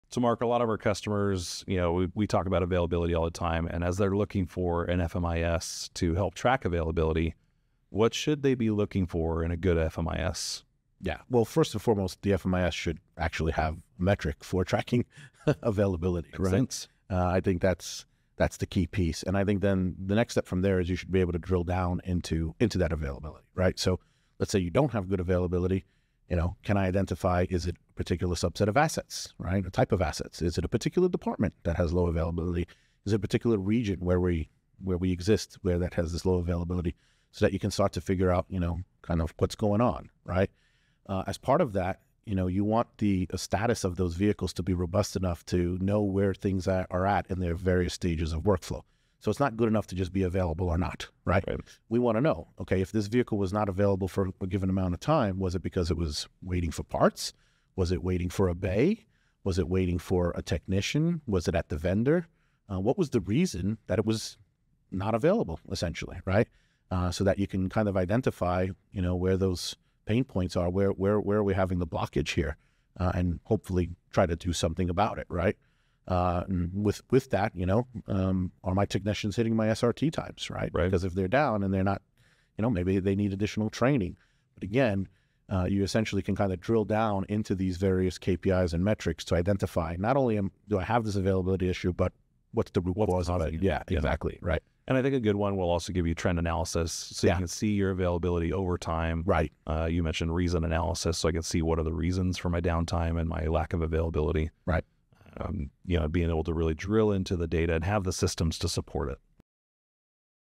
The RIGHT Way to Pick Fleet Management Software
🔑 Fleet managers, when selecting a Fleet Management Information System (FMIS), focus on these key features:
- Track Availability: Ensure the system includes metrics for tracking asset availability.
- Detailed Insights: Look for the ability to drill down into availability data—by asset type, department, or region.
- Status Tracking: The FMIS should provide robust status updates on vehicles—whether they're waiting on parts, technicians, or bays.
- Root Cause Analysis: Identify bottlenecks with downtime reasons and performance KPIs.
- Trend Analysis: Monitor availability trends and system performance over time.